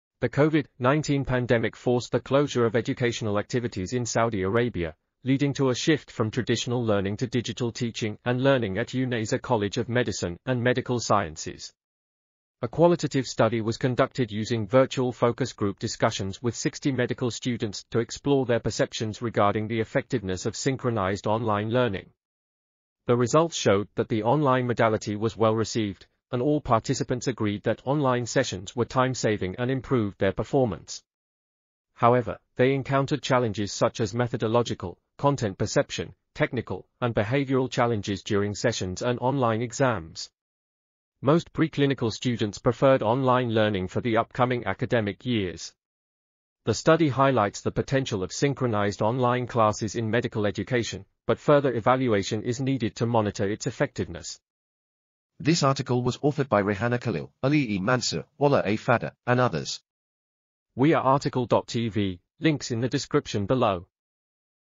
The sudden transition to synchronized online learning during the COVID-19 pandemic in... | RTCL.TV
### Keywords ###

### Article Attribution ###
Title: The sudden transition to synchronized online learning during the COVID-19 pandemic in Saudi Arabia: a qualitative study exploring medical students’ perspectives
Authors: Rehana Khalil, Ali E. Mansour, Walaa A. Fadda, Khaled Almisnid, Mohammed Aldamegh, Abdullah Al-Nafeesah, Azzam Alkhalifah ,and Osama Al-Wutayd
Publisher: BMC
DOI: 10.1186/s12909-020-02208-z

### Video Timestamps ###
0:00:00 - Summary
0:00:55 - Title
0:01:00 - Tail
0:01:04 - End